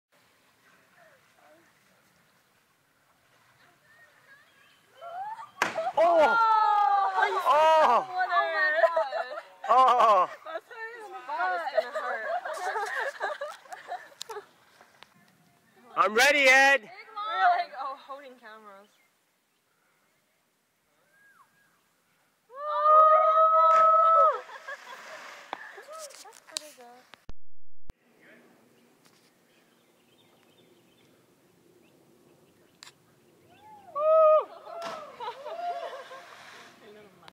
Cliff Jumping @ Great Falls - June '11
WARNING:  DO NOT attempt to swim across the river to get to these jumping locations regardless of your swimming ability.  I am an experienced swimmer and I almost drowned.  I am lucky to be alive.  DO NOT attempt to do these activities.  You are putting your life in danger.